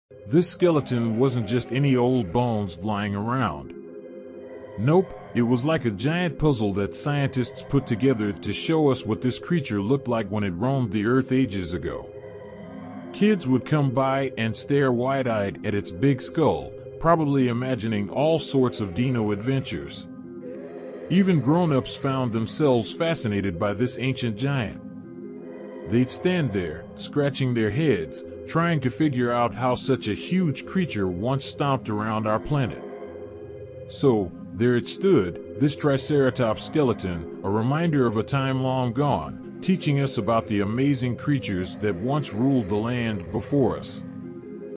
The Toughest Of All Dinosaurs: The Triceratopss
Picture this: a prehistoric showdown between two mighty beasts, the Triceratops and the T-Rex. On one side, you have the T-Rex, towering with its razor-sharp teeth and bone-crushing jaws. On the other, the Triceratops, armed to the teeth with its formidable trio of horns.

Let's take a closer look at the Triceratops. Its skull is a fortress of defense, boasting a massive frill adorned with bony spikes, like a medieval knight's helmet bristling with spikes. But it's not just for show; this frill is a shield against the T-Rex's deadly bites.
And those horns? They're not just for decoration.